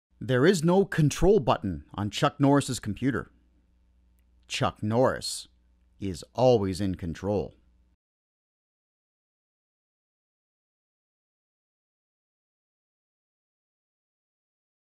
There Is No 'Ctrl' Button On Chuck Norris's Computer. Chuck Norris is Always In Control
Chuck Norris is the king of 80's action movies, and the king of Karate! Chuck Norris Facts are hilarious and they are voiced for you by Kevin Jackal Johnston, the world's greatest human!

CHUCK NORRIS FOREVER! CHUCK NORRIS FACTS!